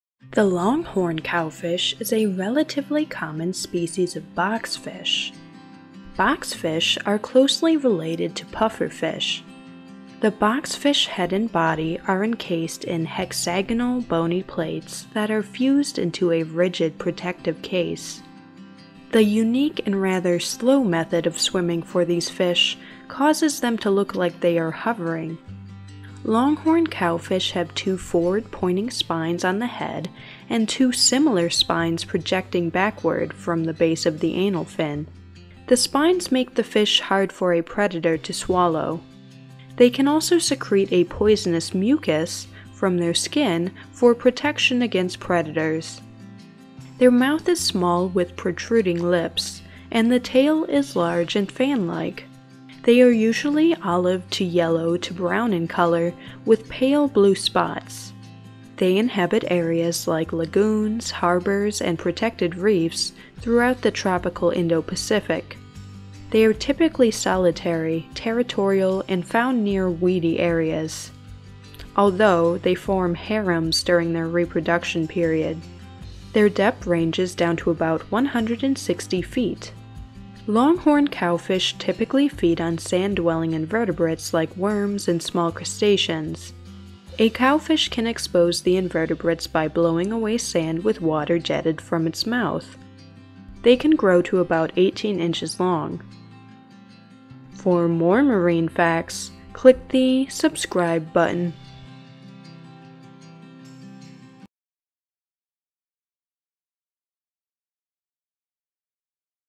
Facts: The Longhorn Cowfish
Quick facts about the longhorn cowfish, an adorable pointy and poisonous boxfish! The longhorn cowfish (Lactoria cornuta, horned boxfish, longhorn boxfish). Longhorn cowfish facts!

King, Dennis. The Reef Guide: Fishes, Corals, Nudibranchs & Other Vertebrates East & South Coasts of Southern Africa. South Africa, Penguin Random House South Africa, 2014.

*All other footage taken by Deep Marine Scenes*